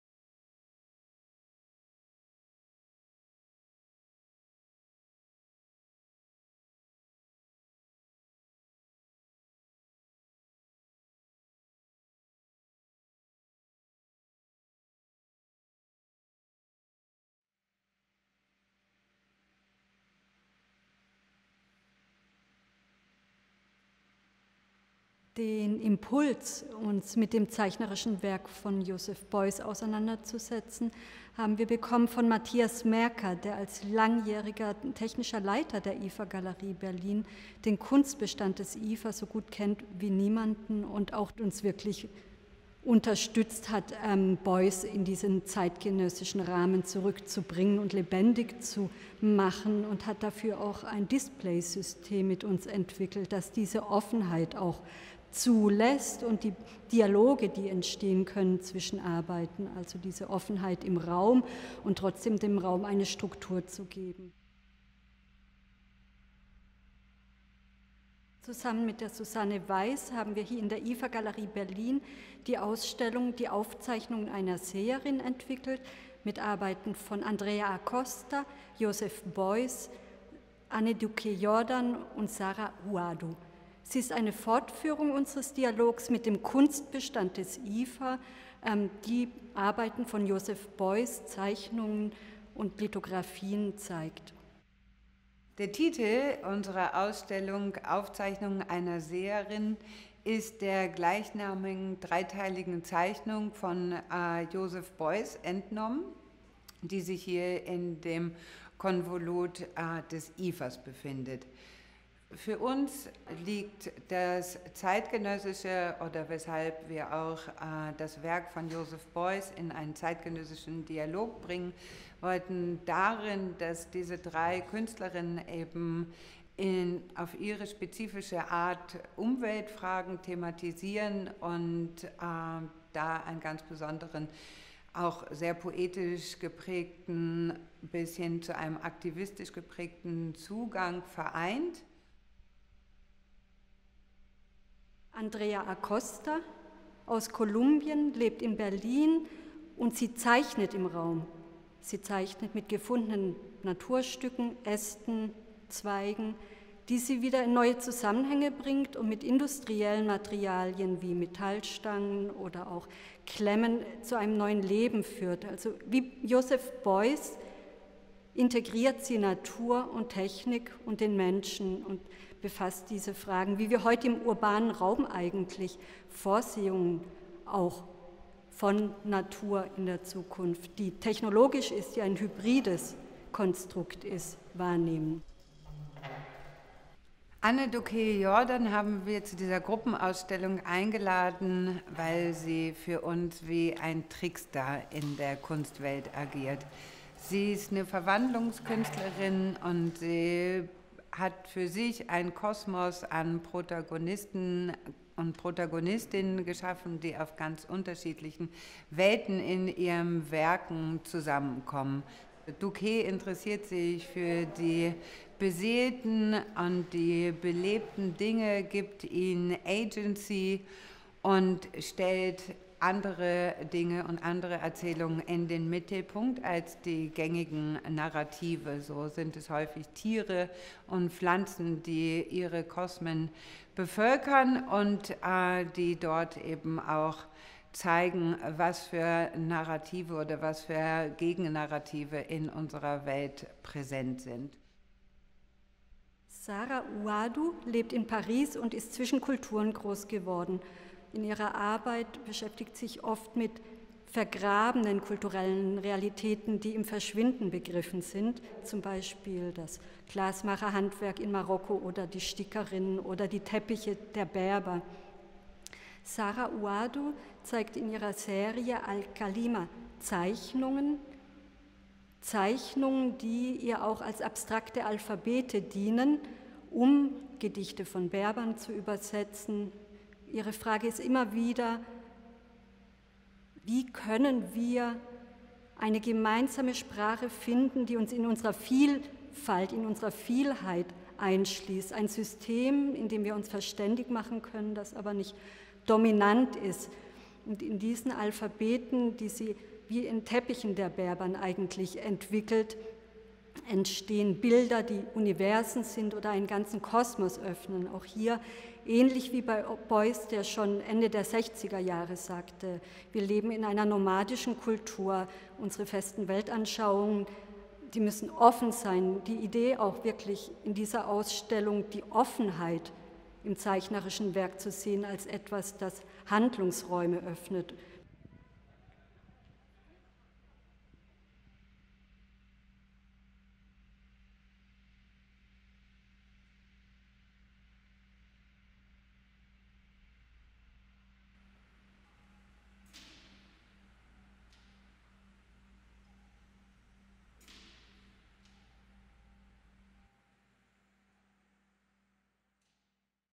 Virtual Tour: Notes of a Seeress
Notes of a Seeress. Andrea Acosta, Joseph Beuys, Anne Duk Hee Jordan, Sara Ouhaddou

30 April – 13 June 2021, ifa Gallery Berlin

To mark the 100th anniversary of his birth, ifa Gallery Berlin is presenting drawings by Joseph Beuys from the ifa collection in dialogue with the artistic positions of Andrea Acosta, Anne Duk Hee Jordan and Sara Ouhaddou. Both Beuys and the featured artists address overlooked, disregarded, handed-down forms of – often collective – knowledge.

Are prophecies akin to interpretations, memories, sketches or schematic images? What do they tell us? Do they unlock new realms in which we can think and act? Such questions form the starting point of the exhibition Notes of a Seeress, which takes its name from a 1958 drawing by Joseph Beuys

Based on the relationship to found materials, they use humorous, poetic and social gestures to examine economic and ecological issues, initiating processes that have the potential to effect lasting societal change. The exhibition unifies four artistic perspectives into a singular spatial and mental cosmos that comments on the social and aesthetic shifts in contemporary society and the potential dialogues they give rise to.

Curated by Susanne Weiß & Inka Gressel

Virtual Tour
Image composition: Victoria Tomaschko
Concept and editing: Victoria Tomaschko and Alexander Gheorghiu
Original sound: Hannes Schulze